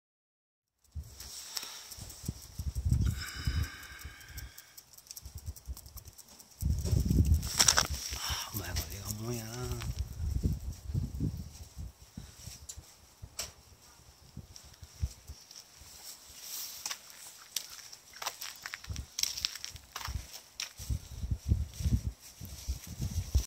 吾輩はカモである？？？？
小屋から解放されて、
しばしのびのびと散策中！
とことことこ、、、、みたいな感じ。